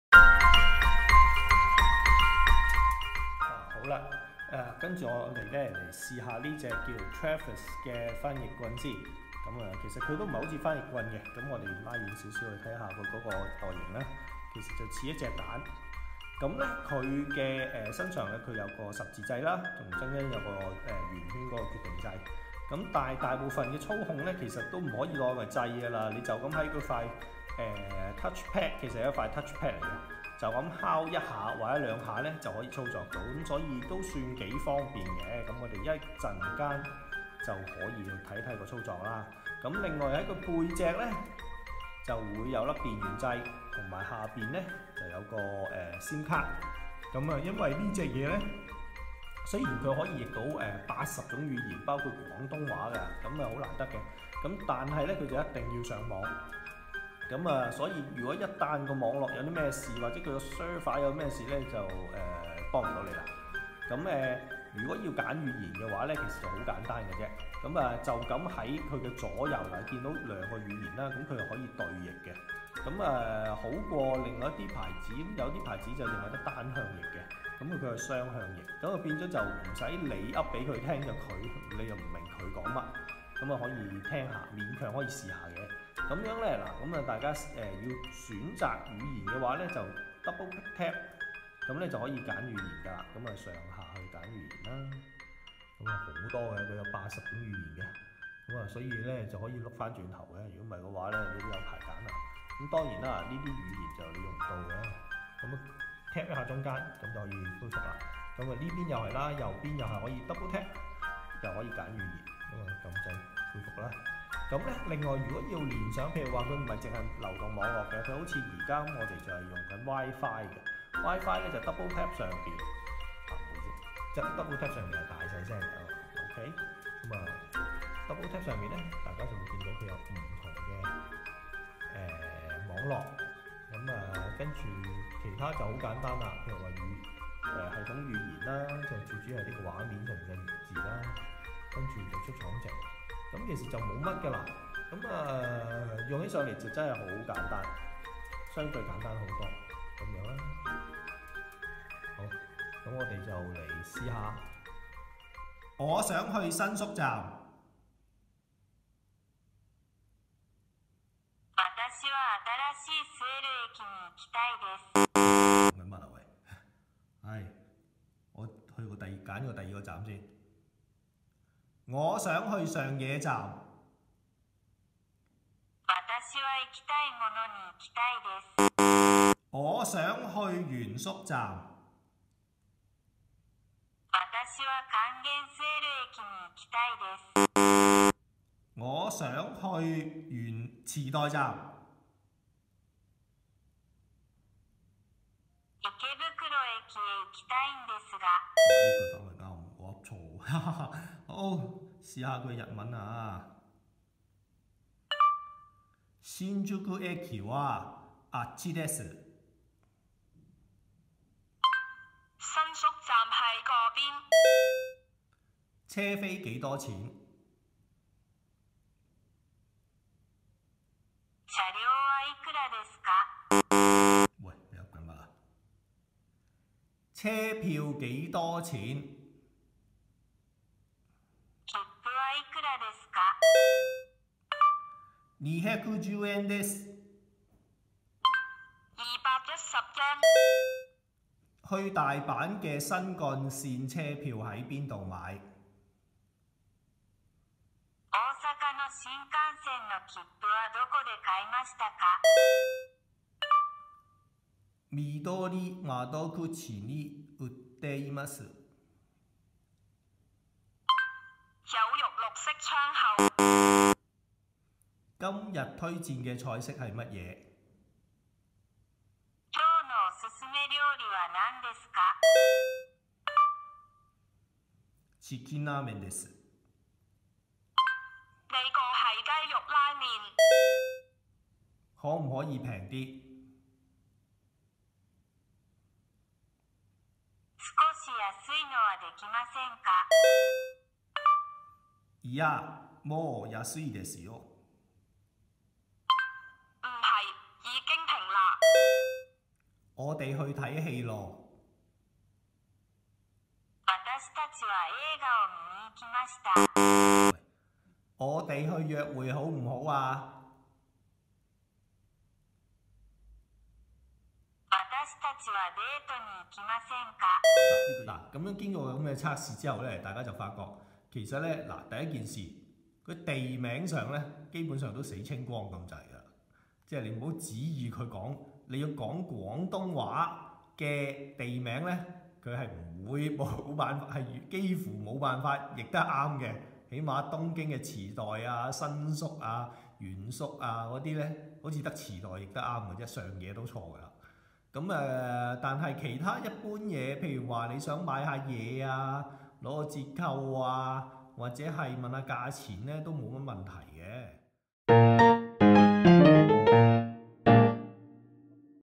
智能翻譯機逐一試 2：Travis 翻譯寶
這一款 Travis 翻譯蛋是測試當中可翻譯最多語言的，最重要的是它支援廣東話輸入，這對香港人來說是最體貼的。另外，它也能夠雙方翻譯，所以基本上可以達成完整的溝通。雖然它也可以進行離線翻譯 20 種語言，但要準確的話還是要連接上網。而連線就可以裝上 SIM 卡直接連上流動網絡、連接 Wi-Fi 或 Hotspot 。